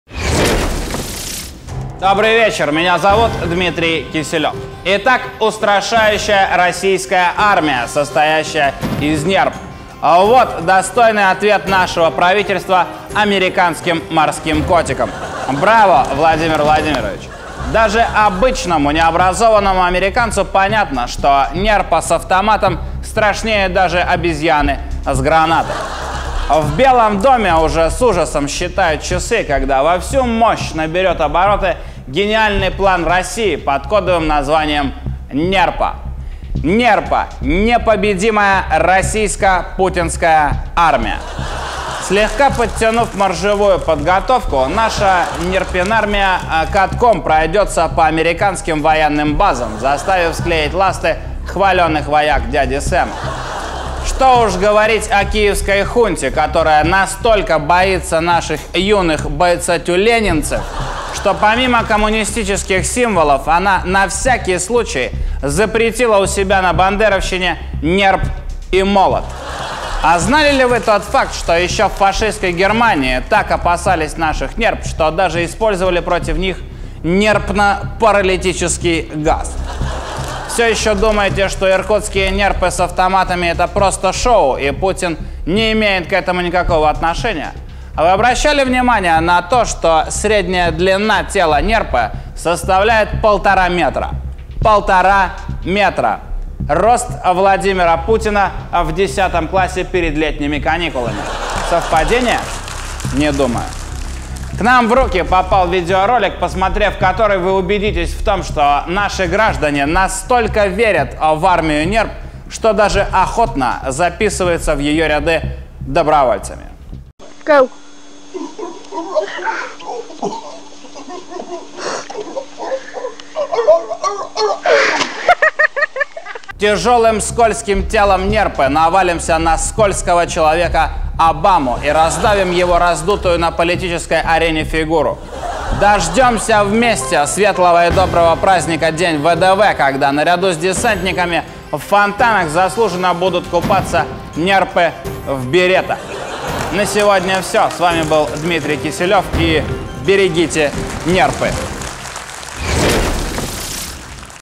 Дмитрий Киселев - НЕРПА|На бандеровщине запретили Нерп и Молот|ответ морским котикам|Новости России
Устрашающая Российская армия, состоящая из НЕРП. Вот достойный ответ американским морским котикам.
НЕРПА с автоматом страшнее даже обезьяны с гранатой.
НЕРПА - Непобедимая Российская Путинская Армия
Киевская хунта настолько боится наших бойцатюленевцев, что помимо коммунистических символов запретила у себя на бандеровщине Нерп и Молот.
Иркутские нерпы с автоматами.

Официальный канал Студии Квартал 95 Контент перероблений для міжнародного використання